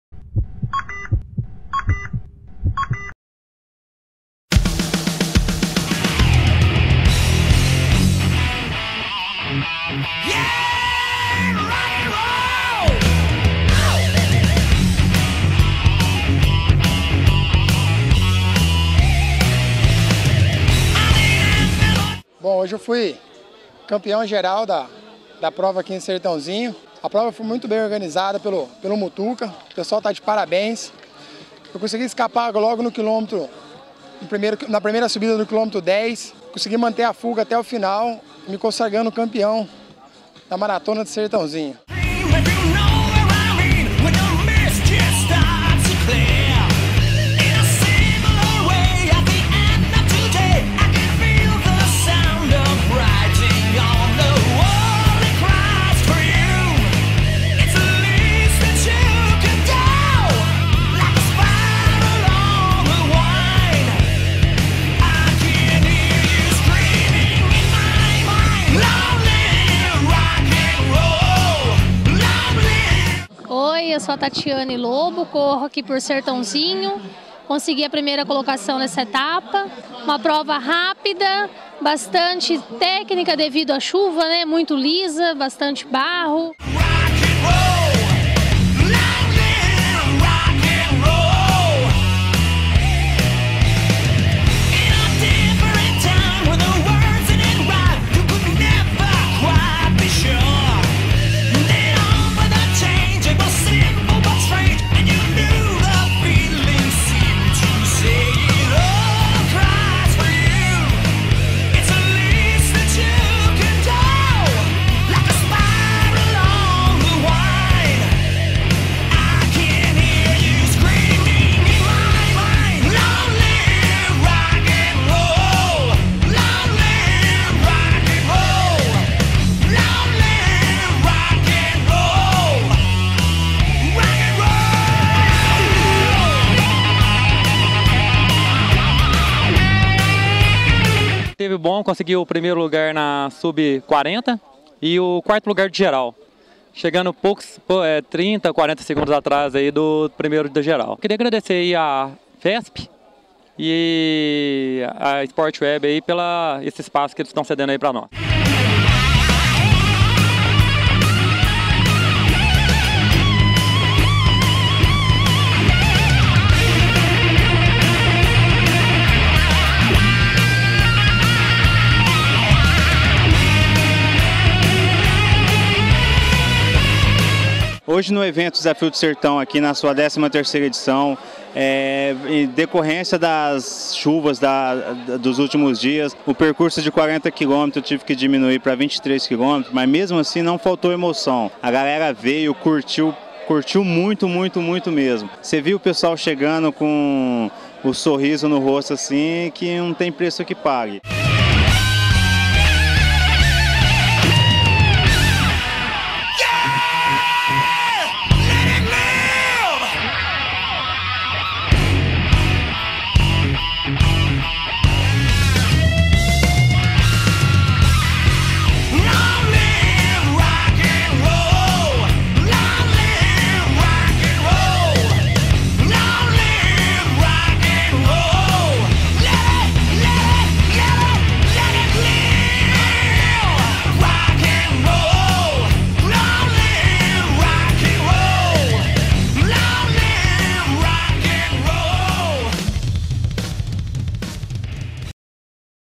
ALIGA MTB CUP - Desafio do Sertão
Evento bem organizado e com recorde de público! Confira o depoimento dos vencedores e as belas imagens do Desafio do Sertão.